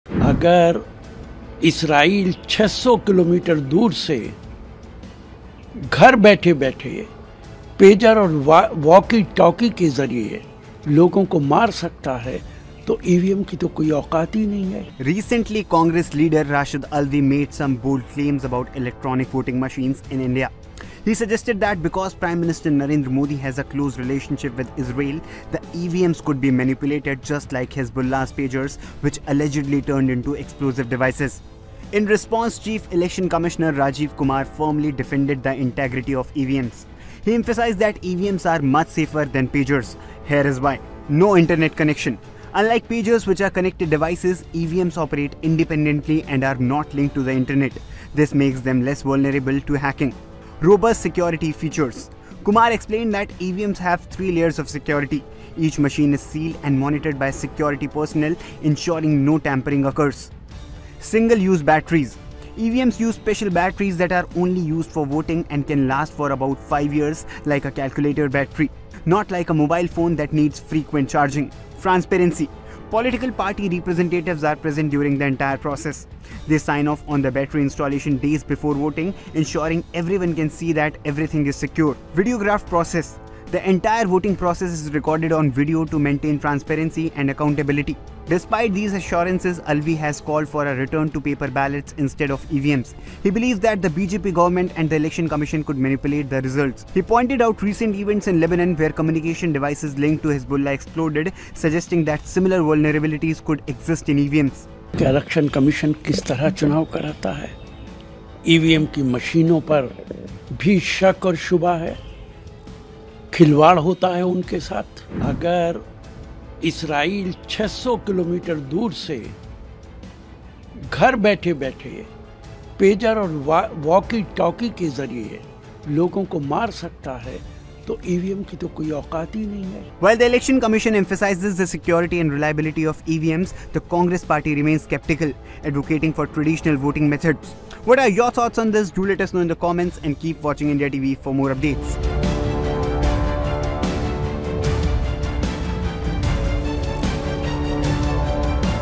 Congress Questions EVMs, EC Steps In to Explain Differences with Pagers
Recently, Congress leader Rashid Alvi made some bold claims about electronic voting machines (EVMs) in India. He suggested that because Prime Minister Narendra Modi has a close relationship with Israel, the EVMs could be manipulated—just like Hezbollah’s pagers, which allegedly turned into explosive devices.